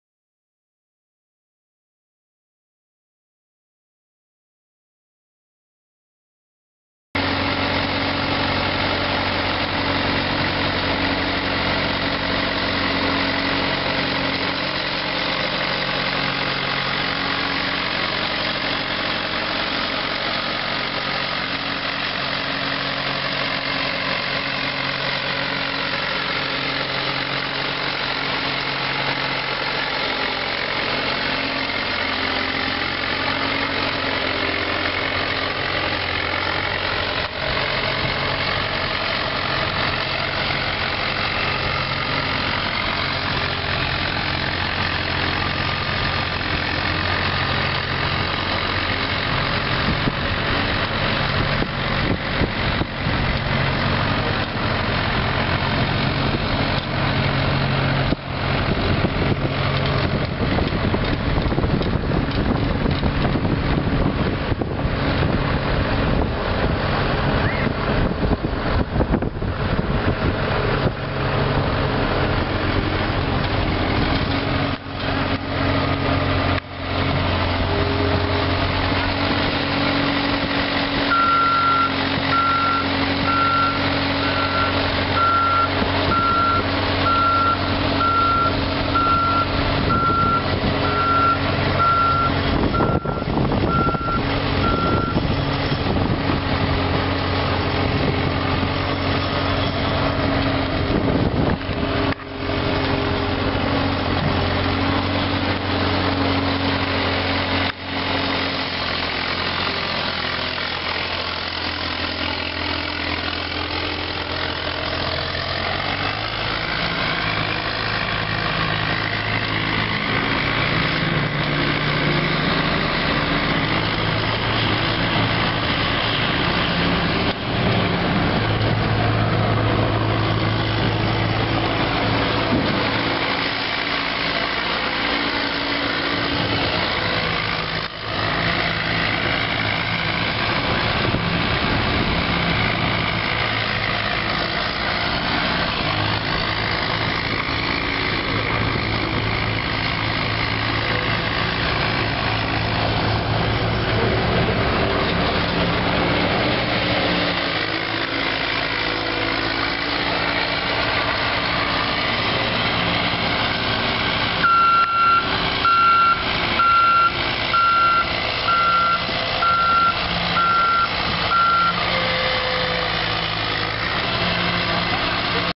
1988 Elgin Pelican Premier street sweeper for sale | sold at auction December 29, 2011
1988 Elgin Pelican Premier street sweeper
16,457 miles on odometer
5,433 hours on meter
John Deere four cylinder diesel engine
Unit# 04FJ192989
Serial 104239T192989
Approx. 6&#039;W sweeper boom
Gutter broom attachment with water applicator broom recently replaced
Enclosed cab
Single operator
AC and heat
Side window "box" viewing extensions
10.00-20 front tires
8.25-15TR rear tires
Cracked windshield
Serial P0262-S